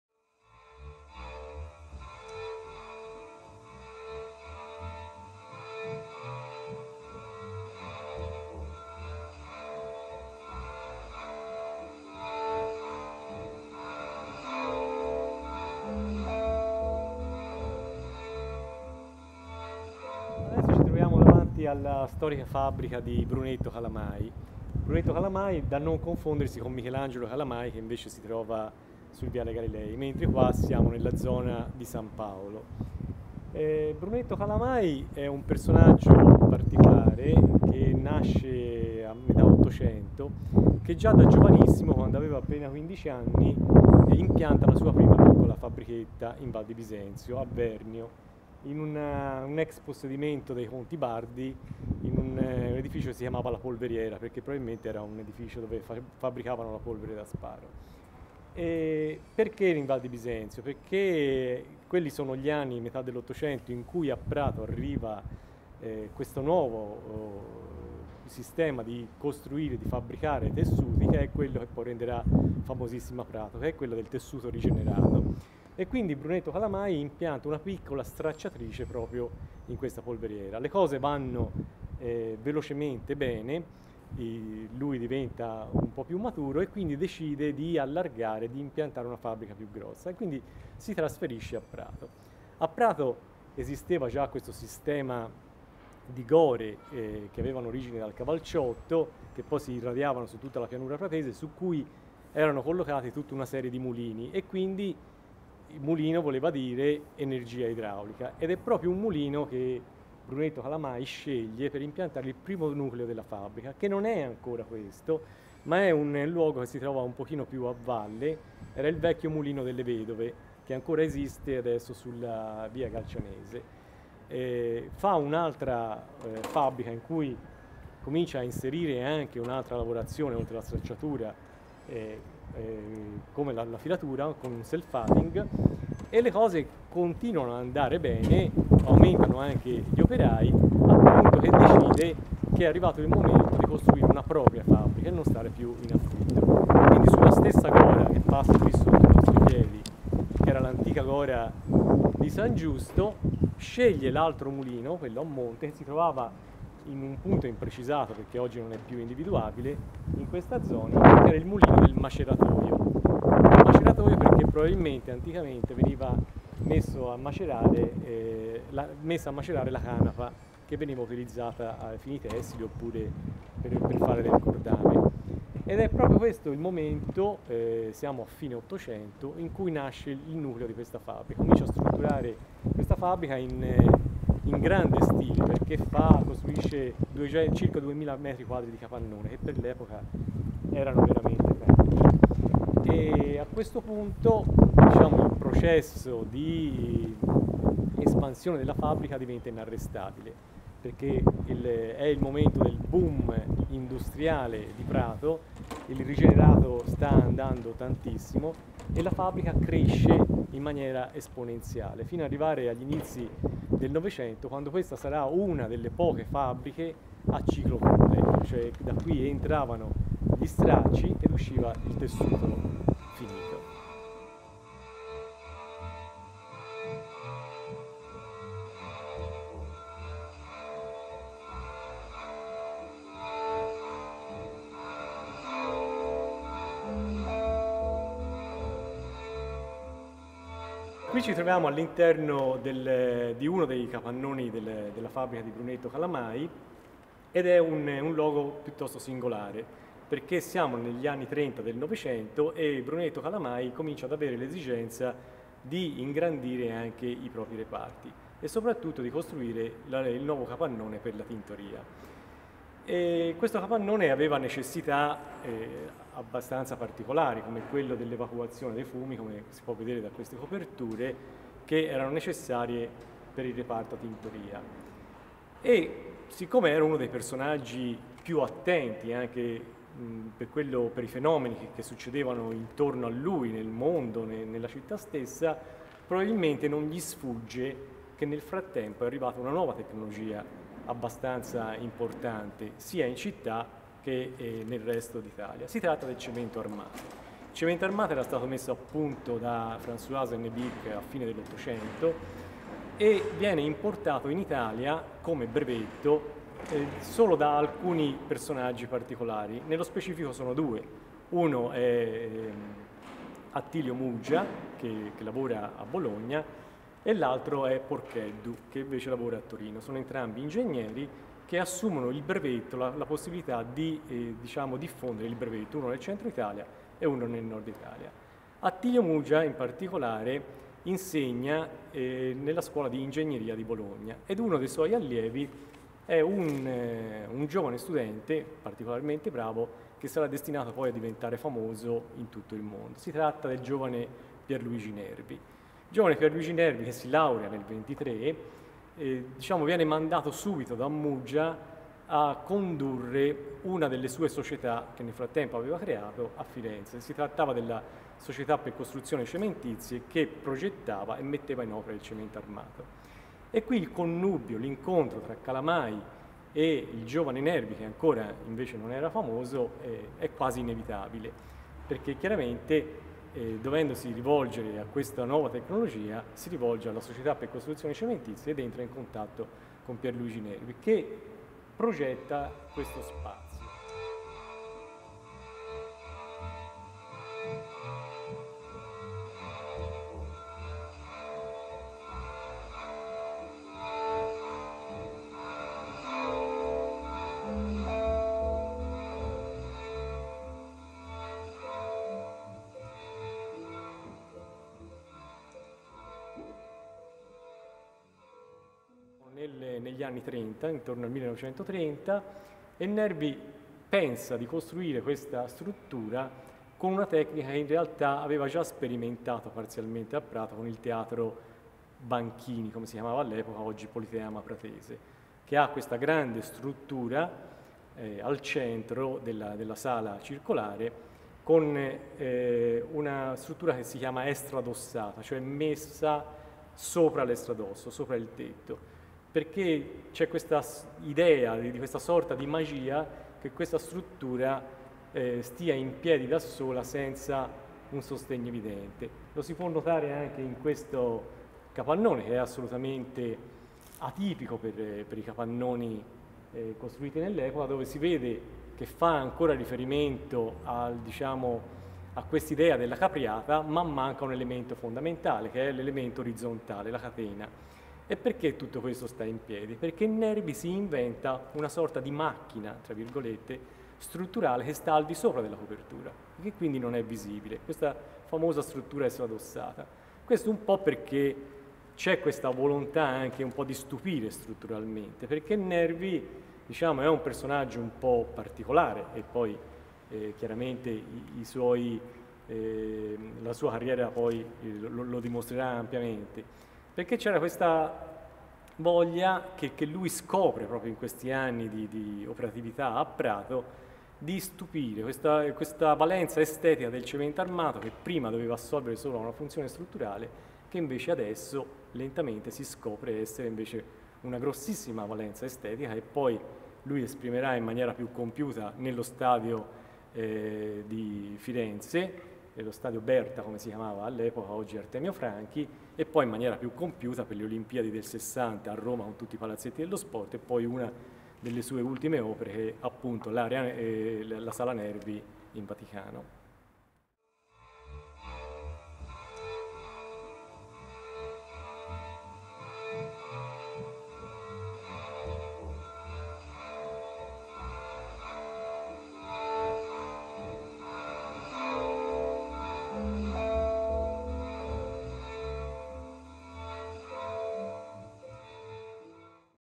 TAI2016 intervista a Giuseppe Guanci / ANONIMA CALAMAI
TAI - Tuscan Art Industry 2016 -  opening 26/11/16
 TAI / DIARI URBANI
 Itinerario di Archeologia Industriale a cura di Giuseppe Guanci.
“La fabbrica di Brunetto Calamai e P.Luigi Nervi a Prato”

La fabbrica di Brunetto Calamai costituisce un'eccezione nel panorama cittadino pratese in quanto è senz'altro tra le fabbriche di più antica formazione, posta sul ramo di una gora, e rimasta quasi intatta fino ai giorni nostri, nonostante sia collocata, com'è, al limite del cosiddetto Macrolotto 0.  La visita quindi sarà particolarmente evocativa, poichè permetterá di leggere una parte della città attraverso le sue varie fasi di sviluppo, sia urbanistico che architettonico, come attestano anche le interessantissime strutture realizzate da un giovanissimo Pier Luigi Nervi, e rimaste per anni completamente sconosciute.